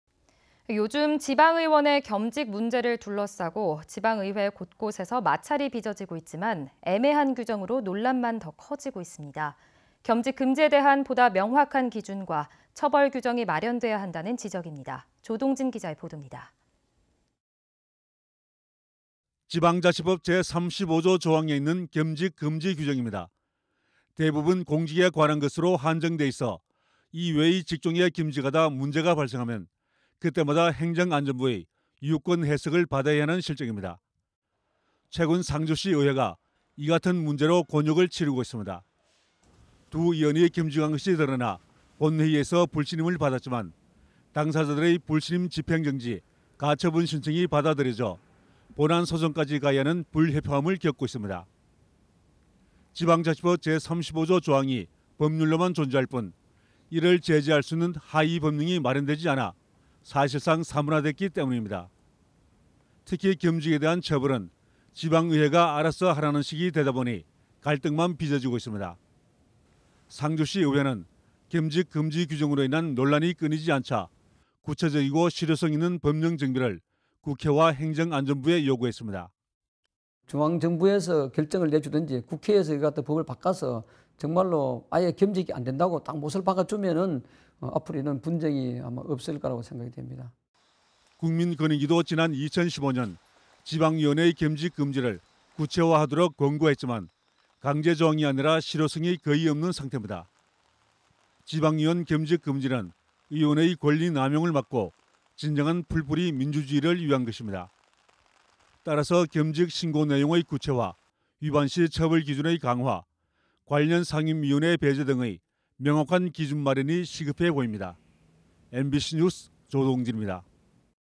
지방의원 겸직 금지 처벌 규정 시급-조동진[포항MBC뉴스]
◀ＡＮＣ▶
요즘 지방의원의 겸직 문제를 둘러싸고
지방의회 곳곳에서 마찰이 빚어지고 있지만
애매한 규정으로 논란만 더 커지고 있는데요.

겸직 금지에 대한 보다 명확한 기준과
처벌 규정이 마련되야 한다는 지적입니다.
     조동진기자의 보도입니다.
           ◀ＥＮＤ▶

           ◀ＶＣＲ▶

(CG)
지방자치법 제35조 조항에 있는
겸직 금지 규정입니다.

대부분 공직에 관한 것으로 한정돼 있어
이외의 직종에 겸직하다 문제가 발생하면
그때 마다 행정안전부의 유권해석을 받아야
하는 실정입니다.  (끝)

최근 상주시의회가 이같은 문제로
곤욕을 치르고 있습니다.

두 의원이 겸직한 것이 드러나
본의회에서 불신임을 받았지만
당사자들의 불신임 집행정지 가처분신청이
받아 들여져 본안소송까지 가야 하는
불협화음을 겪고 있습니다.

지방자치법 제35조 조항이 법률로만 존재할 뿐
이를 제재할 수 있는 하위 법령이 마련되지
않아 사실상 사문화됐기 때문입니다.

특히 겸직에 대한 처벌은
지방의회가 알아서 하라는 식이 되다 보니
갈등만 빚어지고 있습니다.

상주시의회는 겸직 금지 규정으로 인한
논란이 끊이지 않자 구체적이고 실효성이 있는
법령 정비를 국회와 행정안전부에 요구했습니다.
           ◀ＩＮＴ▶
        정재현 의장 -상주시의회-
"중앙정부에서 결정을 내주던지 국회에서 법을
바꿔서 정말로 아예 겸직이 안된다고 못을
박아주면 앞으로 이런 분쟁이 없을 거라고
생각됩니다."

국민권익위도 지난 2015년
지방의원의 겸직 금지를 구체화하도록
권고했지만 강제조항이 아니라 실효성이
거의 없는 상태입니다.

지방의원 겸직 금지는 의원의 권리 남용을 막고
진정한 풀뿌리 민주주의를 위한 것입니다.

따라서 겸직 신고 내용의 구체화,
위반시 처벌 기준의 강화,관련 상임위원회
배제 등 명확한 기준 마련이 시급해 보입니다.
     mbc news 조동진입니다.